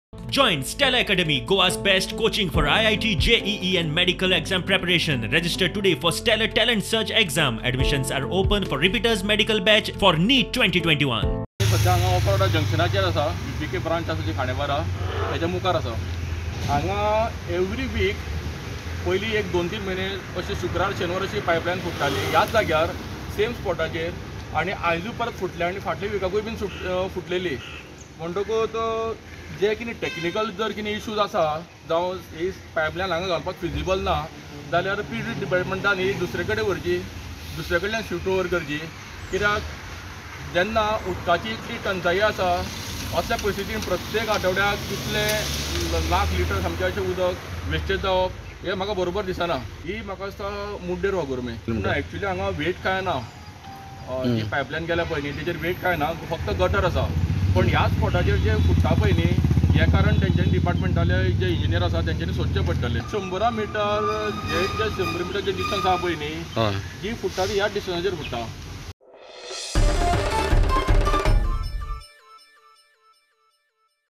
OPA TO MUDDER WATER PIPELINE BURSTS; THOUSANDS OF LITRES OF WATER WASTED
Water pipeline from Opa to Mudder has again developed a crack due to which thousands of litres of water has got wasted. The locals have said that the pipeline at this particular slot again and again breaks down. The locals have demanded shifting of the said pipeline. Due to the pipeline bust, the market area of Khandepar and Mudder area has gone dry.